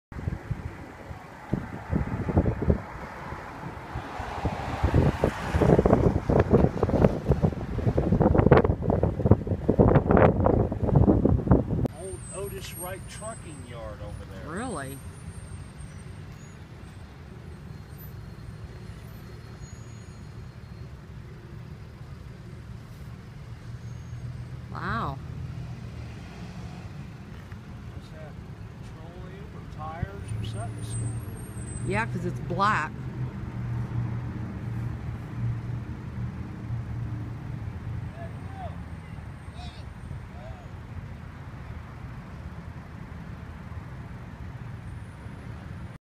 Fire
Otis Wright Trucking Yard Building On Fire in Lima, Ohio. October 07, 2014